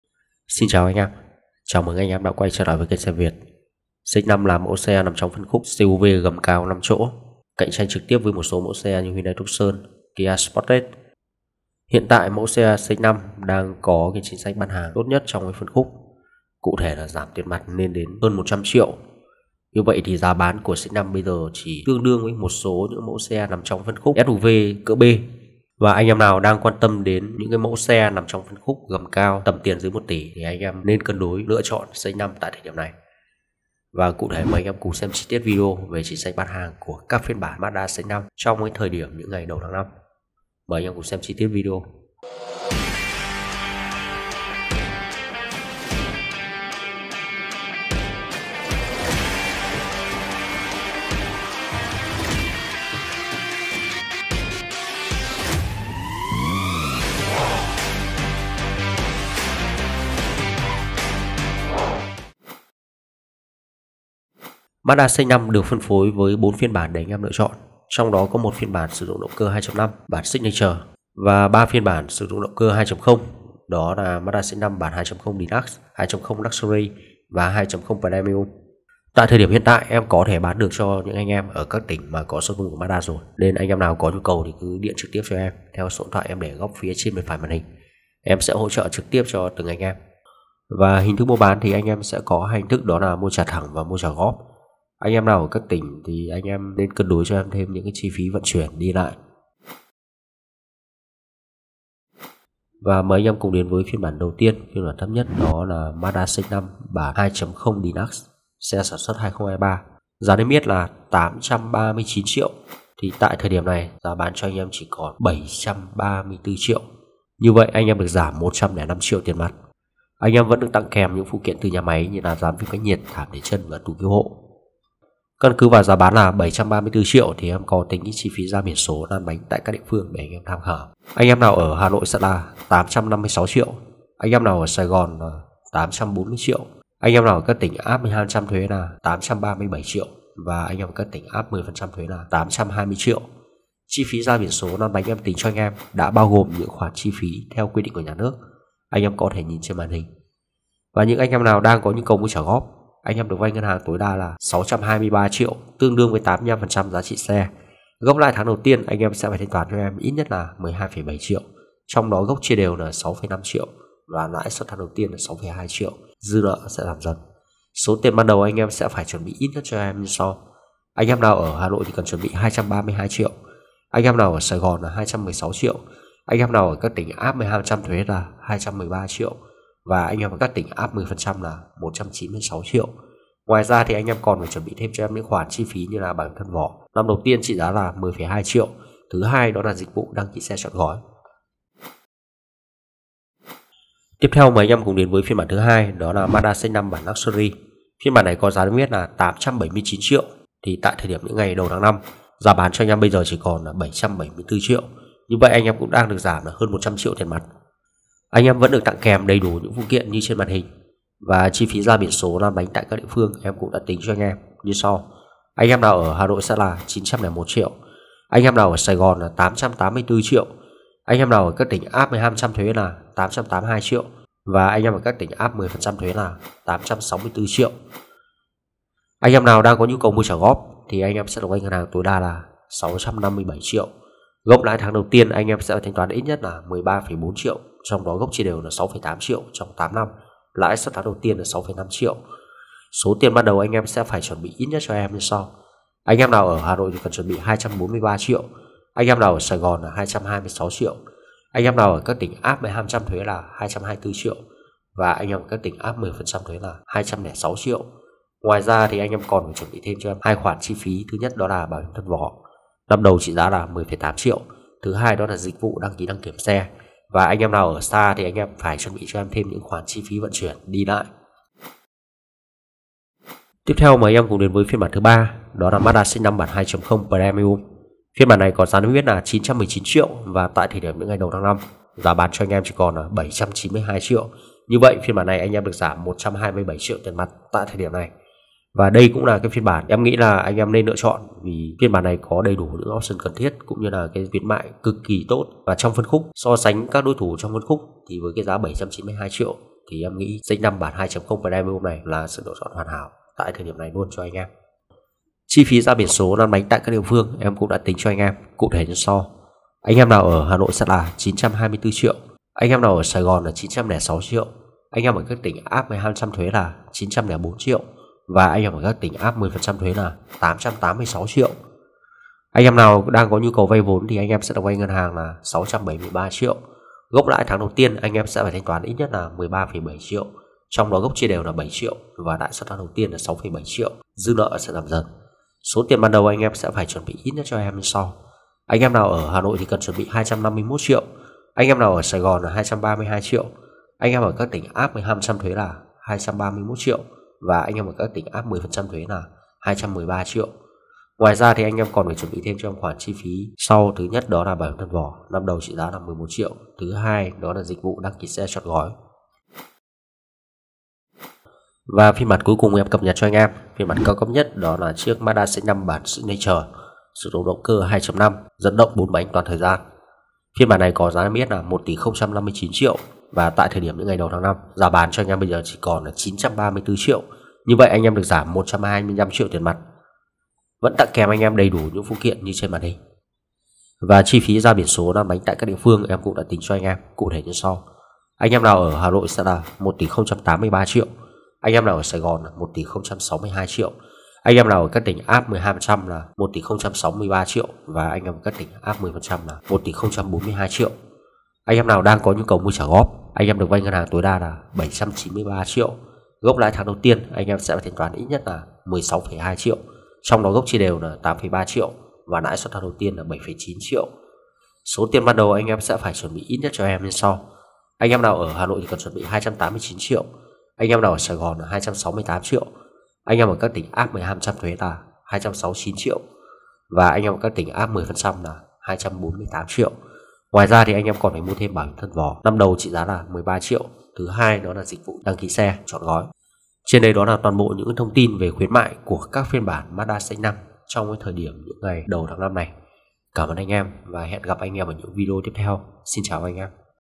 Giá xe Mazda CX5 tháng 05/2023. Khuyến mại tiền mặt nhiều nhất trong phân khúc CUV 05 chỗ.
Cập nhật giá xe Mazda CX5 tháng 05/2023.
Khuyến mại tiền mặt nhiều nhất trong phân khúc CUV 05 chỗ.